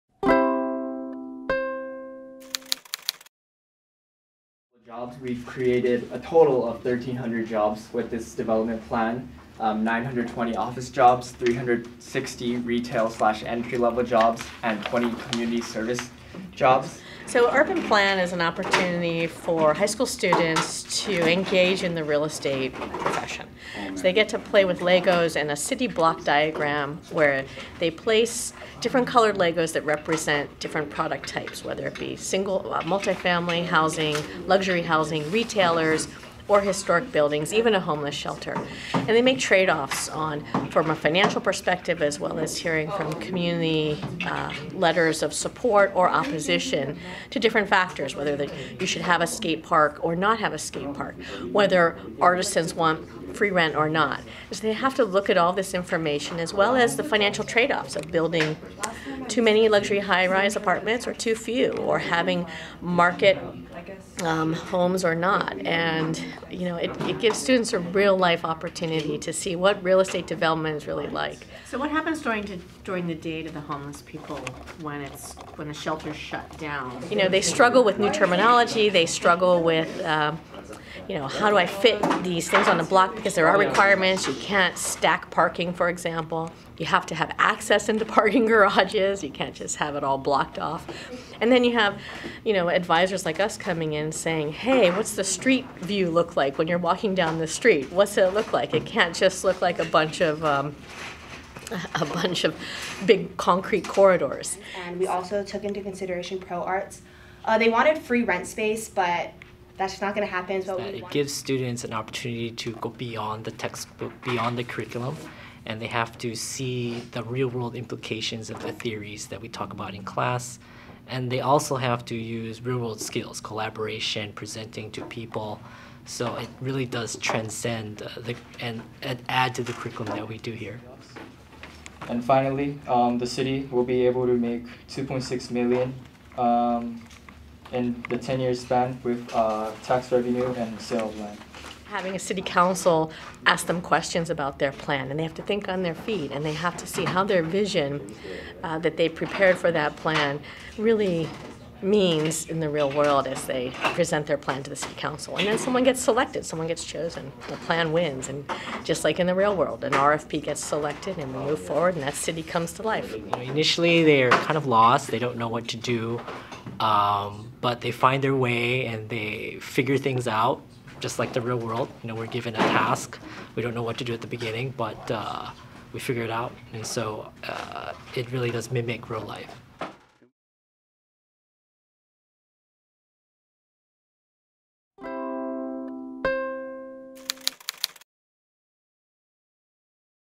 AP Economics students from 'Iolani School participate in urban planning competition
Students from Mr. Lance Suzuki's AP Economics class at 'Iolani School in Honolulu are competing in an urban planning contest to see if they can provide the best revitalization plan for Honolulu. (Video: Dennis Oda)